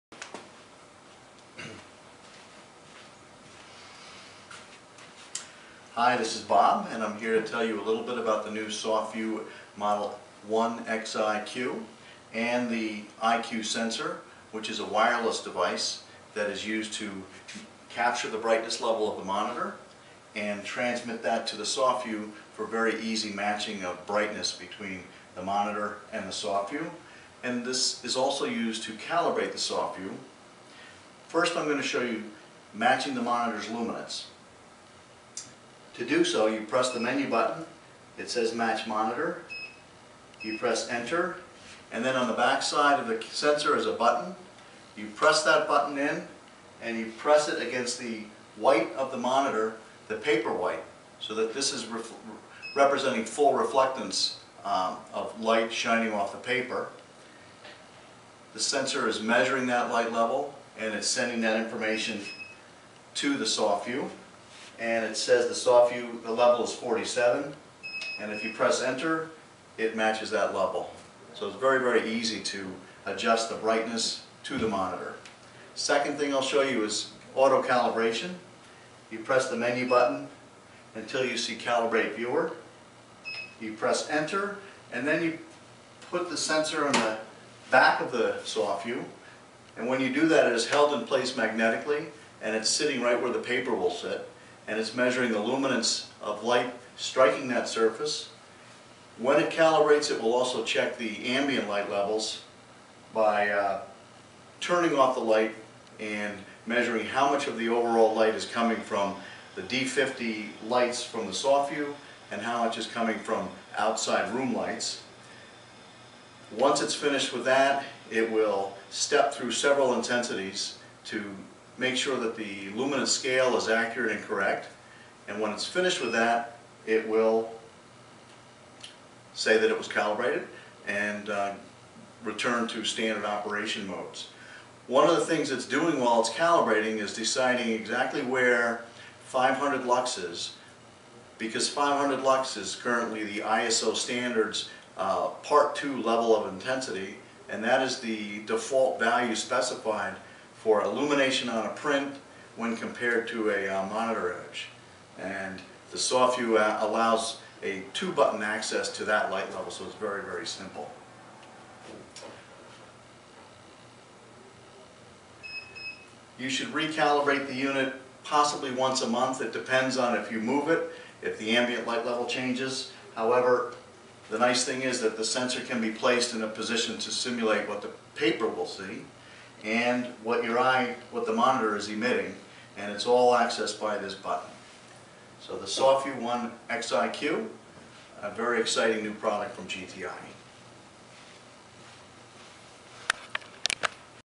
Introducing the Softview SOFV 1xiQ Desktop
This video provides a quick demo and overview of the SoftView SOFV-1xiQ. The Soft View SOFV-1xiQ is a desktop soft proofing system that provides accurate color comparisons between hard copy prints and computer monitors. It includes a viewing station and the revolutionary wireless iQ sensor. The 1xiQ enables superb lighting uniformity with a small desktop footprint that meets ISO 3664:2009 D50 light quality for superior color rendition.